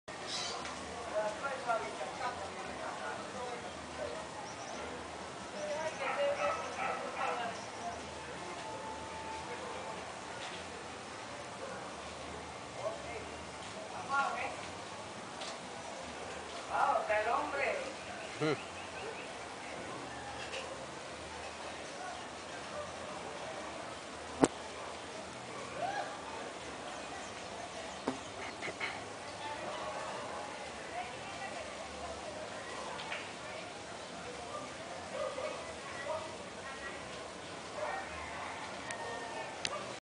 Leafhoppers talking
This leafhoppers is talking but is difficult understand because your idiom is more complicate for the species.